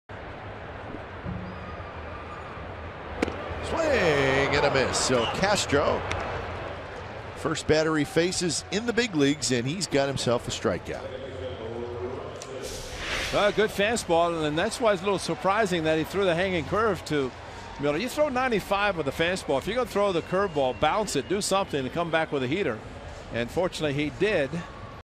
OAK@SEA: Castro fans Miller for first career K
5/9/15: Angel Castro strikes out Brad Miller swinging for his first Major League strikeout in the 6th inning